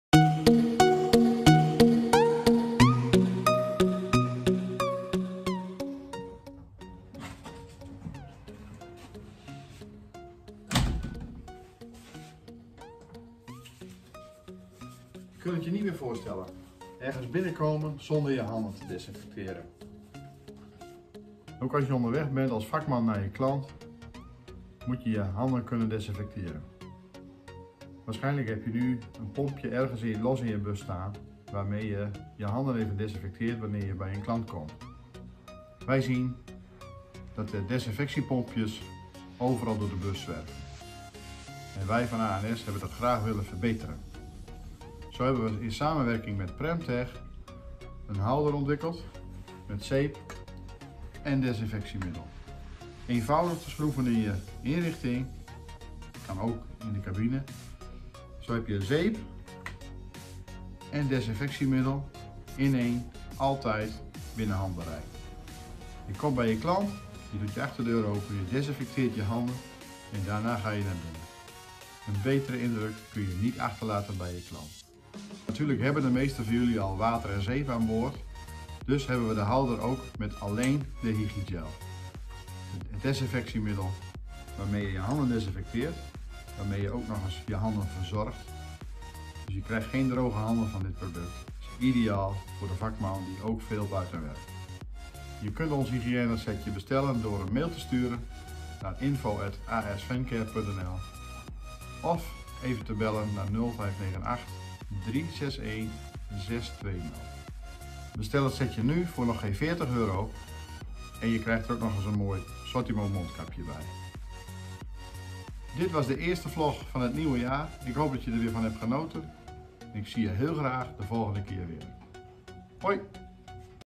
Je handen desinfecteren achter in je bus.
in deze nieuwe vlog willen we je kennis laten maken met ons handige desinfecteer setje voor in je bedrijfsauto. Maak een goede indruk bij je klant en laat zien dat je gezondheid belangrijk vindt!